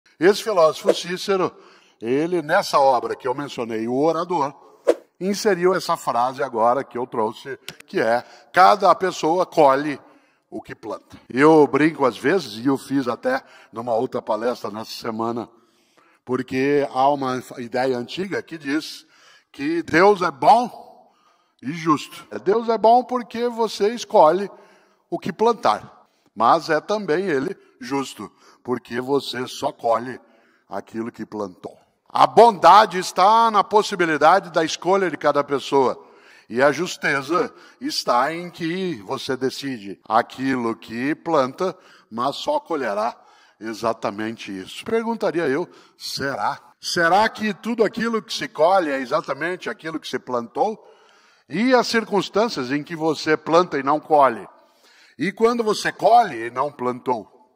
Cada um colhe o que planta - Mario Sergio Cortella
A frase “cada um colhe o que planta” é atribuída ao filósofo Cícero (106-43 a.C.). Ela é comumente associada à ideia cristã de que “Deus é bom e justo”. Bom porque cada um é livre para plantar o que quiser. Mas é justo, porque você só vai plantar o que tiver colhido. Será?